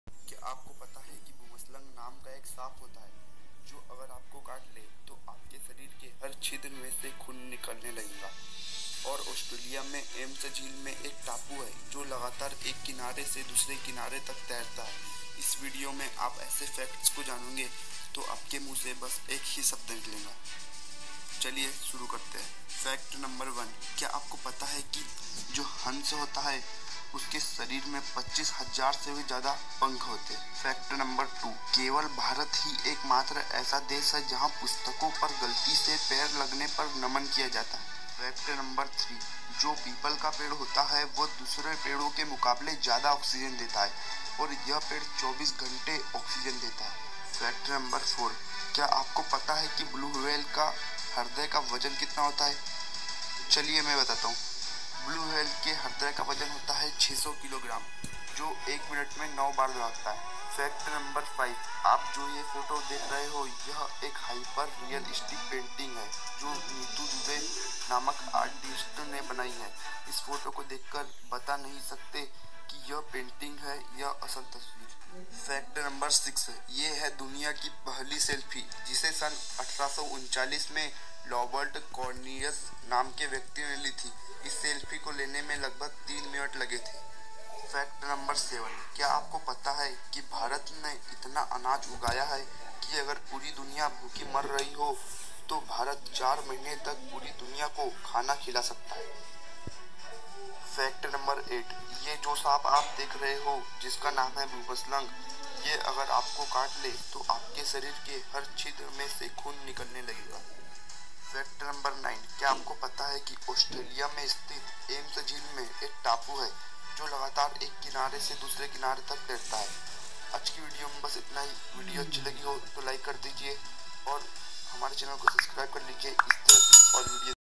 यह टापु तैरता है पानी मे😱😱
यह टापू पानी पर तैरता है
ऑस्ट्रेलिया में स्थित एम्स झील में एक टापू है
जो पानी पर तैरता है
lndian fact takars
ISI tarah ki all video dekhne ke liyeयह टापु तैरता है पानी मे😱😱